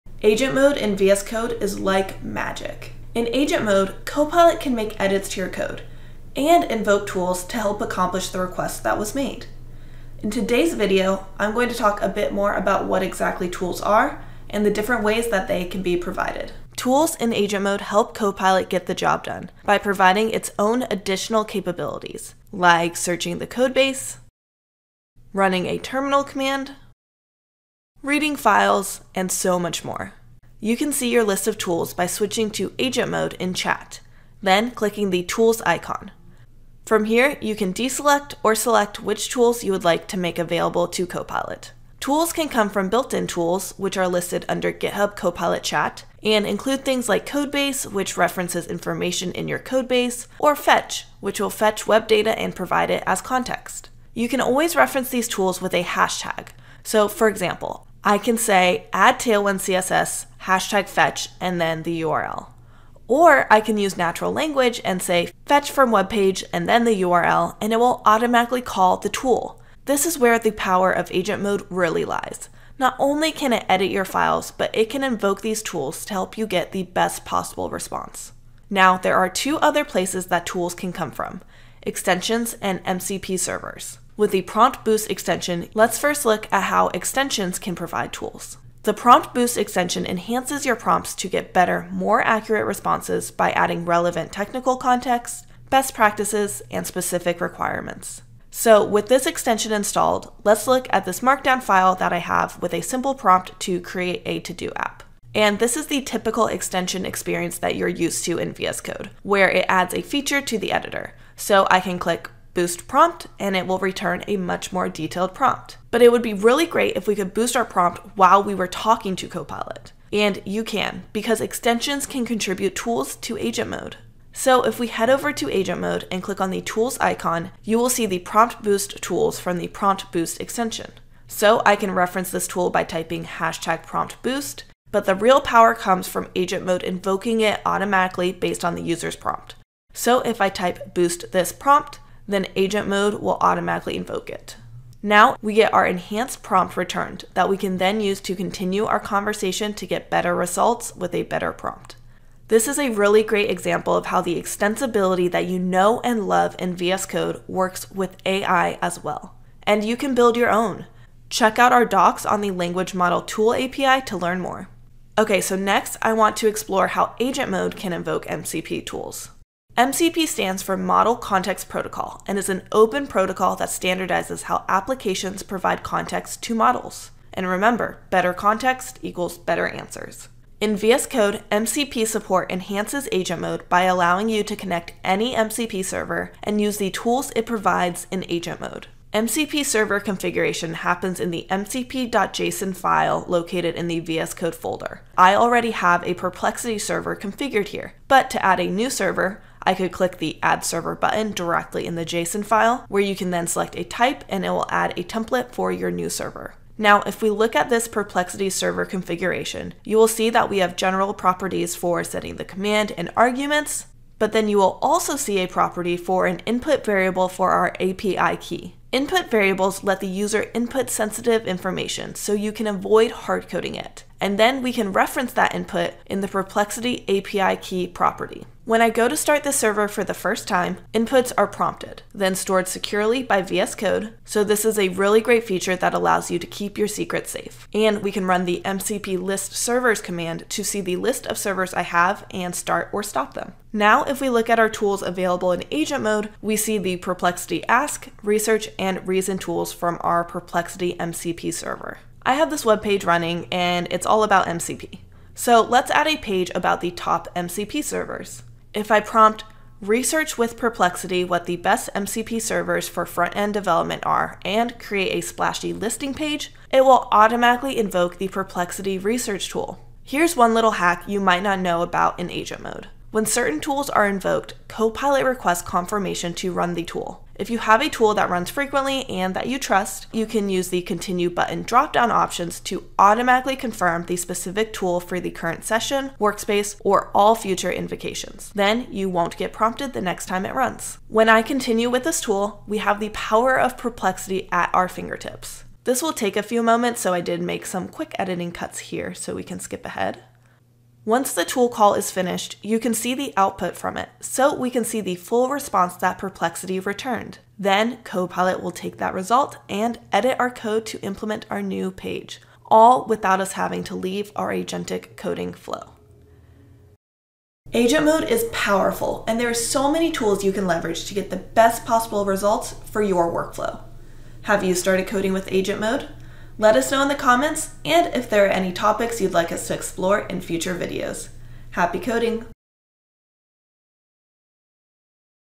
Agent mode tools in VS Code
Agent mode can invoke tools to help accomplish a user's request  - but what exactly does that mean? In this video, we'll go over what agent mode tools are and how they can be provided in VS Code.

00:00 Intro
00:16 What are tools?
00:42 Built in tools
01:24 Tools from extensions - Prompt Boost
02:25 MCP tools - Perplexity MCP server